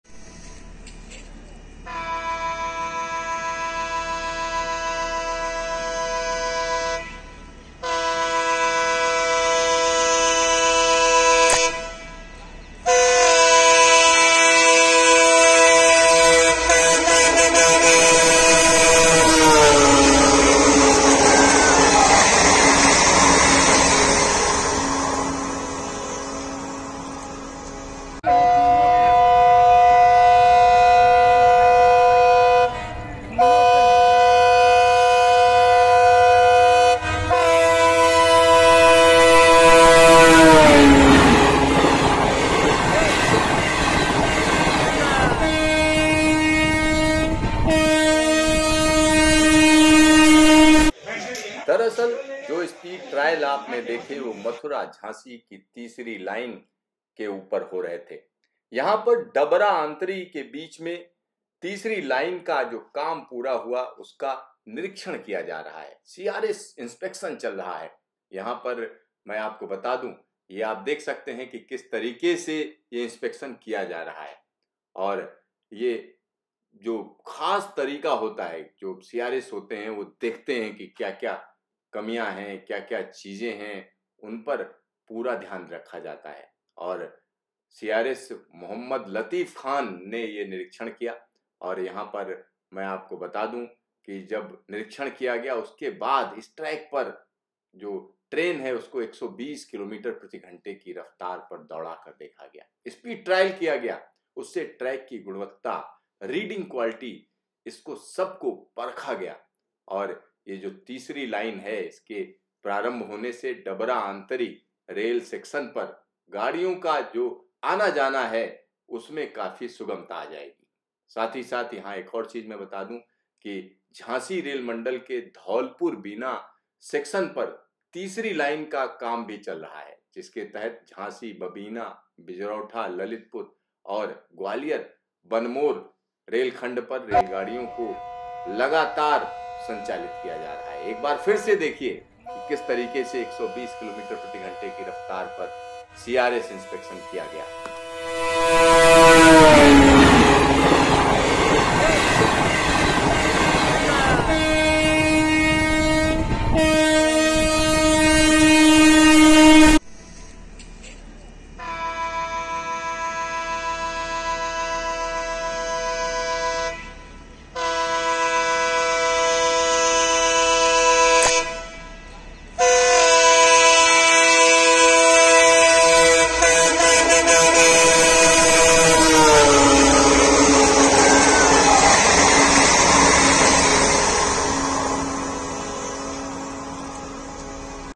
मथुरा झांसी तीसरी रेलवे लाइन परियोजना के तहत डबरा आंतरी के बीच CRS ट्रेन का स्पीड ट्रायल
कमिश्नर आफ रेलवे सेफ्टी मोहम्मद लतीफ खान ने नई बनी ब्रॉडगेज तीसरी रेलवे लाइन डबरा आंतरी का निरीक्षण किया
मथुरा- झाँसी तीसरी लाइन परियोजना के अंतर्गत डबरा-आंतरी के मध्य (19.896 किमी) रेल खंड पर तीसरी लाइन के पूर्ण कार्य का निरीक्षण किया गया, जिसके अंतर्गत ट्रैक्, सिग्नल, गेट, OHE, ब्रिज, पॉइंट्स आदि का गहन निरीक्षण किया गया| इस दौरान उन्होंने इंस्पेक्शन कैरिज तथा मोटर ट्राली से रेलखंड के अंतर्गत आने वाले डबरा,अनंत पैठ एवं आंतरी स्टेशन पर सभी प्रकार के नए संस्थापनों तथा नये स्टेशन भवनों की परख की| साथ ही डबरा- आंतरी रेल खण्ड पर मोटर ट्रॉली से भी  निरीक्षण किया | इसके साथ ही उनके द्वारा एल॰एच॰एस-396 तथा खण्ड के मेजर ब्रिजों का भी विस्तृत निरीक्षण किया गया।